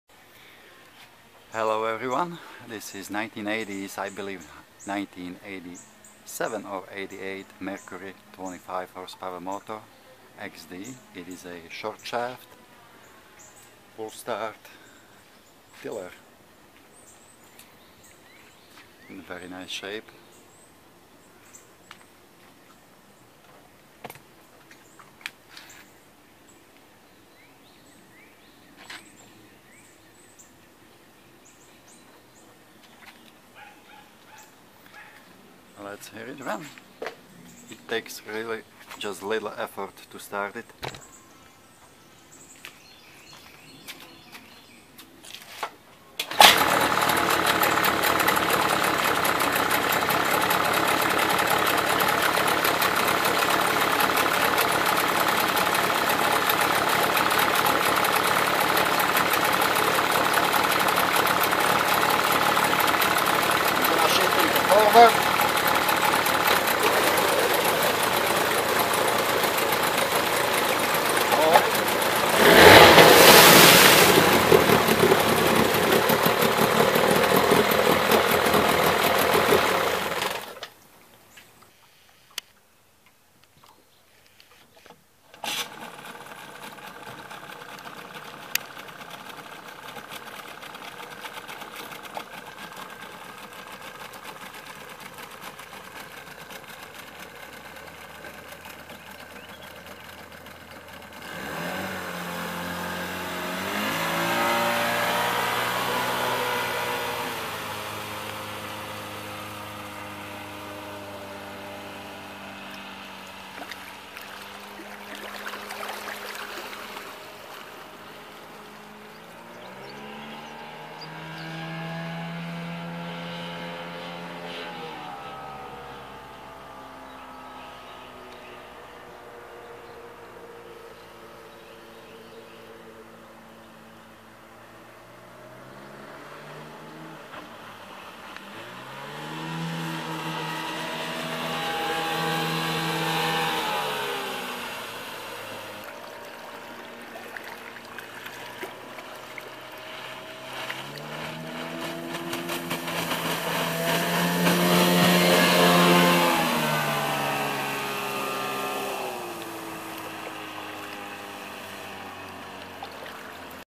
Lake test of Mercury 25XD outboard.
Plenty of power for this little boat. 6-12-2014, lake Allatoona.